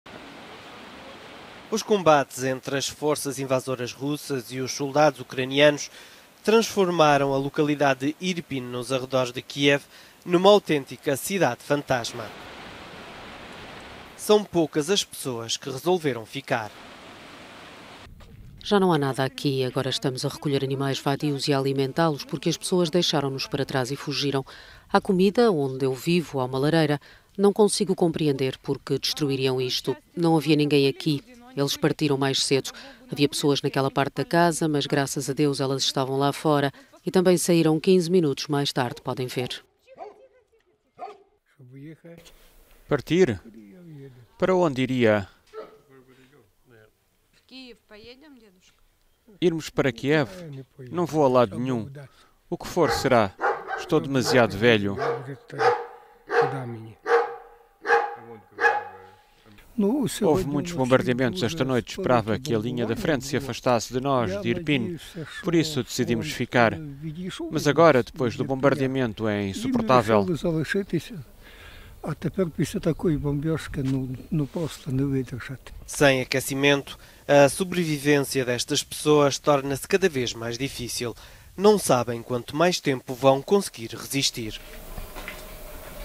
Os resistentes de Irpin
Grupo de ucranianos de Irpin recusa fugir e enfrenta a destruição e o frio